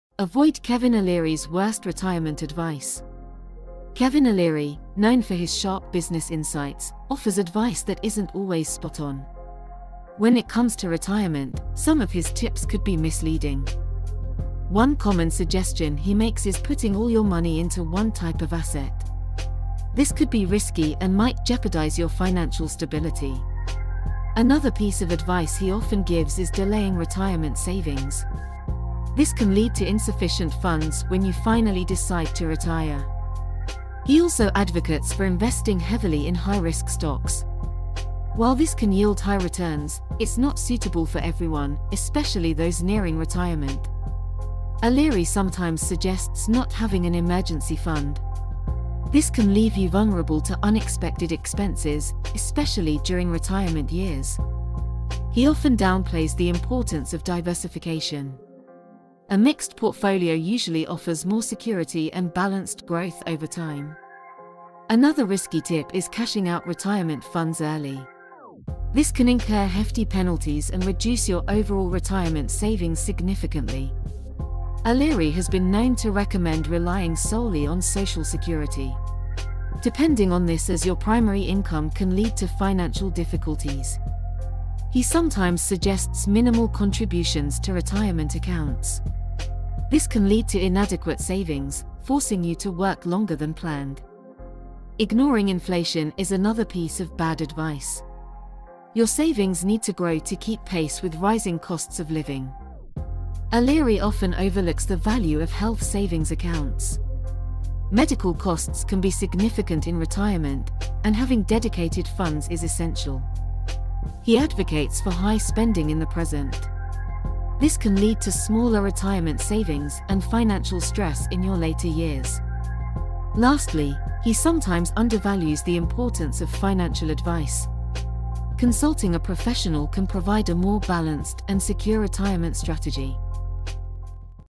Kevin O'Leary's WORST Retirement Advice - AVOID These Mistakes!
In this eye-opening video, we dissect Kevin O'Leary's WORST retirement advice that could jeopardize your financial future! From what stocks to buy, dividend investing, and bitcoin. 🛑 If you're planning for retirement, don't fall into these common traps and mistakes that many investors overlook in their financial literacy. From misunderstood investment strategies to poor savings habits, we reveal critical insights that can help you secure a comfortable retirement through personal finance.

Join us as we break down O'Leary's advice and highlight the essential strategies YOU should be implementing instead. Whether you're just starting your career or nearing retirement age, this video is packed with valuable tips to ensure you build a solid financial foundation.